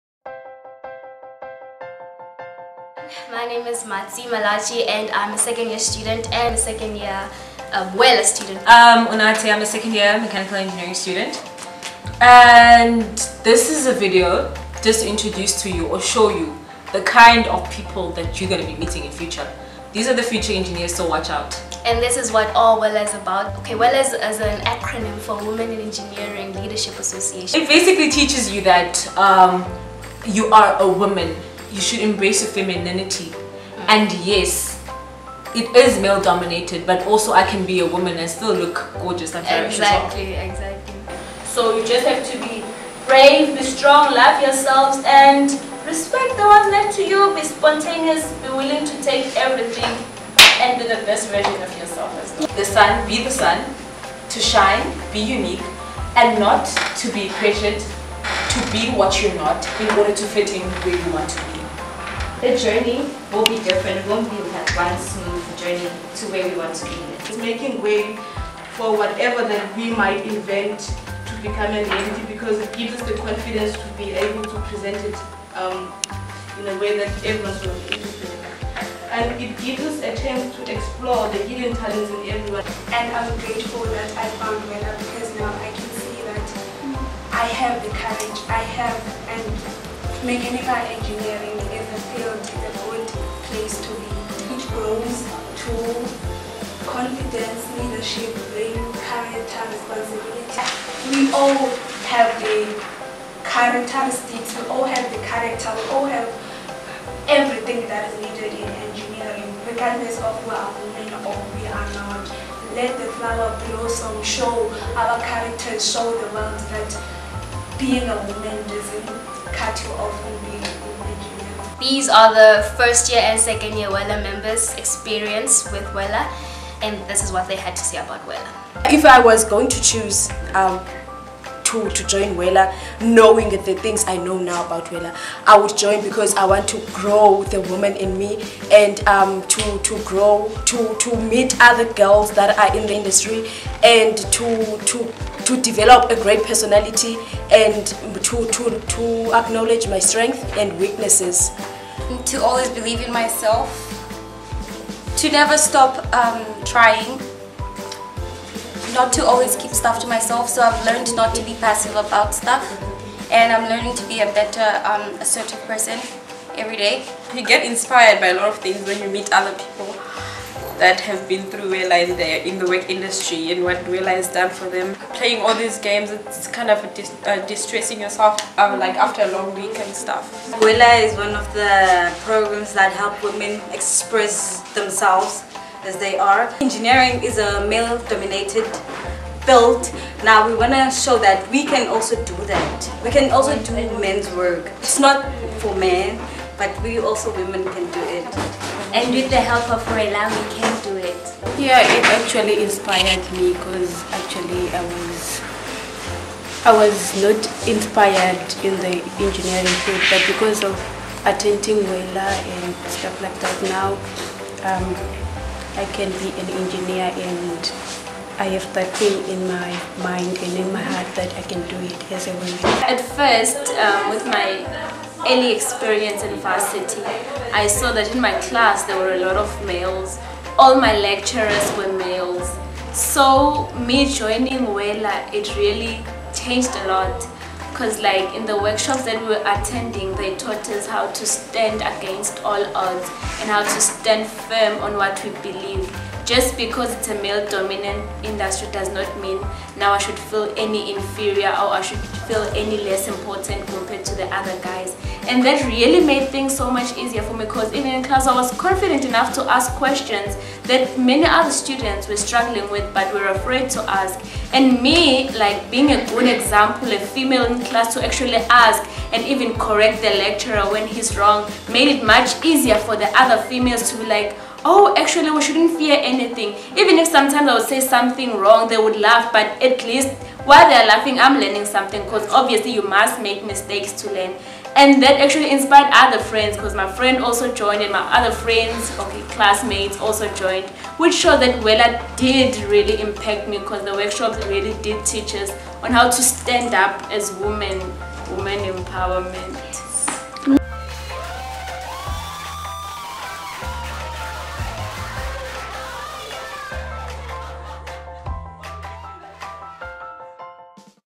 Women in Engineering Leadership at Mandela University
WELA merSETA (Women in Engineering Leadership Association) aims to support women in the Engineering field. Our goal is to empower the Engineering students of Nelson Mandela University – develop, grow and expand their abilities confidently through our programme. This video shows the members’ experience of WELA.